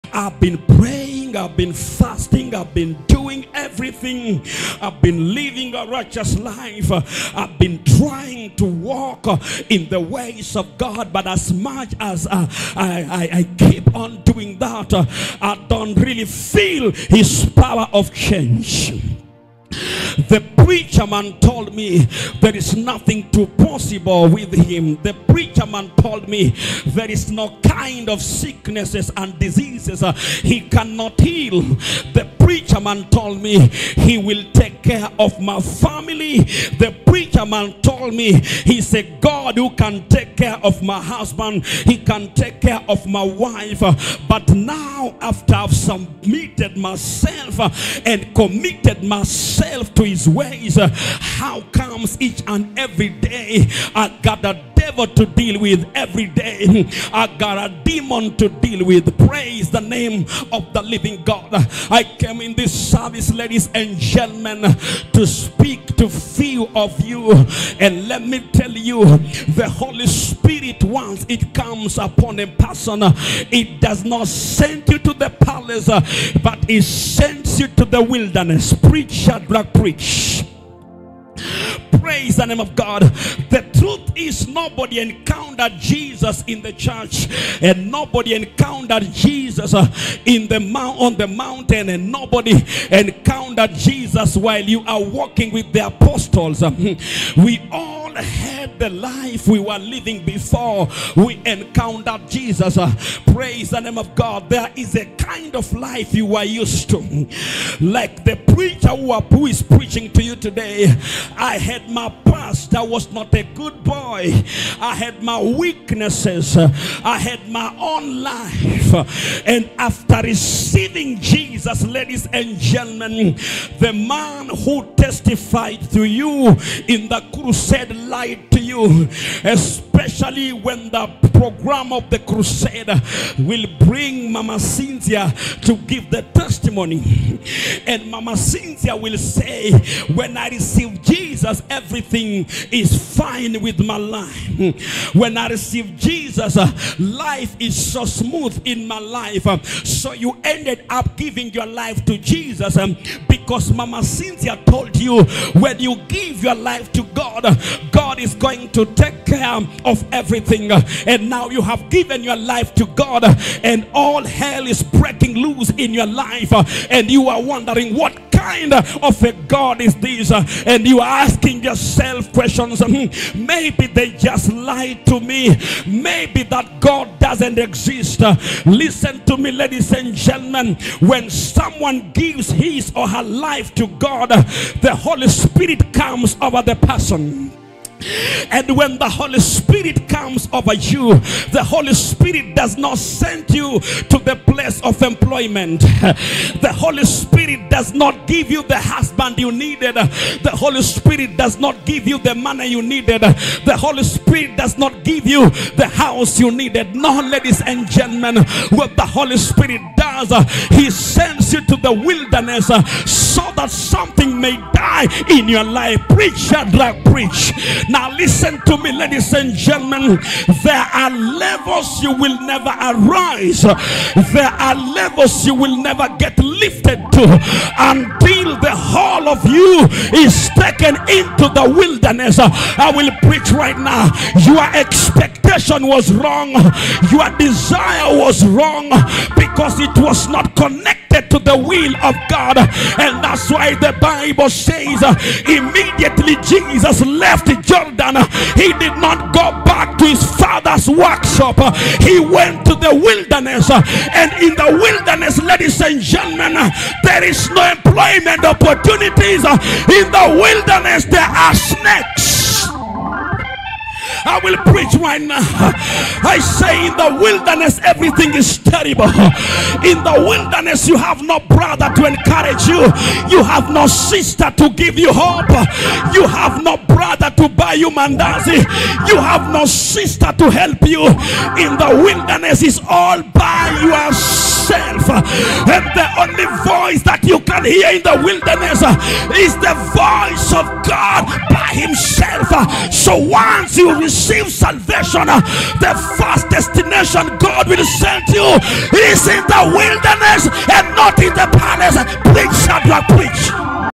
THE WILDERNESS EXPERIENCE
When you encounter Jesus Christ the Holy spirit does not take you to a palace but He takes to the wilderness, so that something may die in the you.